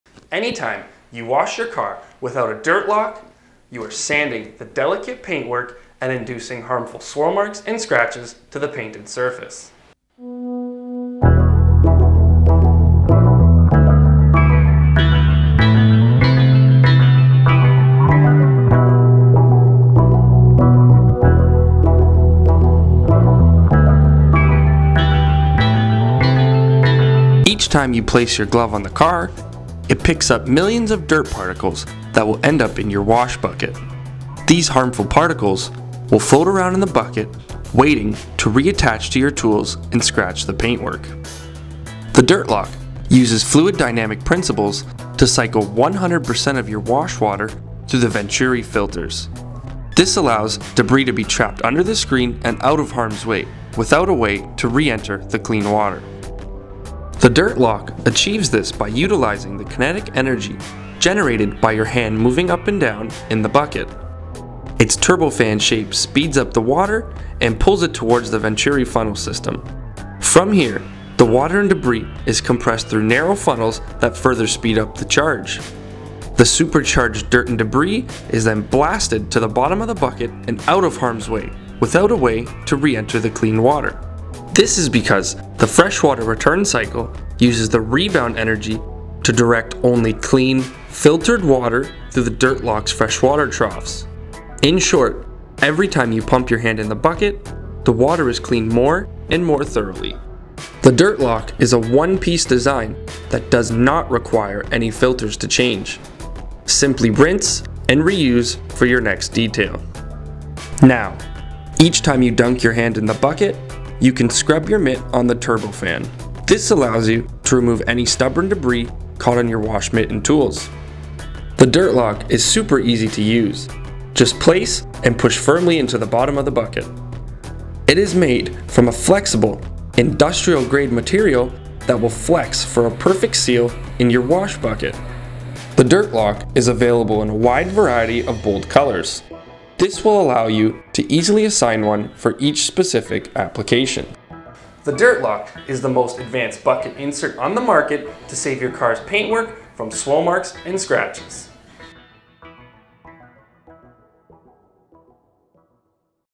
Turbine Dirt Lock Car Wash Bucket Insert - The Detail Guardz Car Care
The Turbine Dirt Lock is the bucket insert that WON'T disappoint. It's the bucket filter that was engineered for dirt extraction.

🆕***UPDATE 2020 THE DIRT LOCK HAS RECEIVED SOME WICKED UPGRADES OVER THE YEARS. IT NOW FEATURES AUTOMATIC SELF-TENSION ADJUSTMENT AND A NEW MATERIAL THAT MAKES THE PRODUCT SINK LIKE AN ANCHOR. THIS MEANS THE DIRT LOCK WILL FIT PERFECTLY INSIDE NEARLY EVERY 3-8 GALLON BUCKETS WITHOUT ANY ISSUES!***🆕

The Dirt Lock flexible bucket insert blasts dirt and grime to the bottom of the bucket with the force of your hand. It essentially acts as a pump to power and force dirt to the bottom of the bucket and out of harms way, without a way to re-enter the clean water.  It's the result of perfect engineering utilizing fluid dynamics and the Venturi effect. This is essential for a scratch-less car wash.

The other products on the market have not grasped the concept of utilizing fluid dynamics to make the bucket insert act as a pump. Our patented design utilizes the motion of your hand to filter nearly 100% of your wash water. Each time you pulse your hand, the water is dramatically sped up by the shape of the turbine and through hundreds of tapered Venturi funnels. The compression of the water and debris passing through the turbine & venturi funnels will blast dirt to the bottom of the bucket, with only fresh, clean water returning along the side of the dirt-lock through the fresh water troughs.

In-short, every time you pump your hand up and down, you are cleaning the water more and more thoroughly.

--- KEY FEATURES
- Manipulates the flow of water, allowing debris to be trapped under the screen
- Tapered filters cycle dirt particles underneath the filter and clean water above
- Flexible rubber tabs lock the unit in the bucket
- Automatic self adjusting tension for easy install / removal in nearly all buckets
- Solid industrial plastic makes the unit sink like an anchor
- Chemical & crush resistant
- Rinse & re-use
- Dirt Lock pad washer attachment add-on feature

*MADE IN CANADA FOR LONG-LASTING DURABILITY*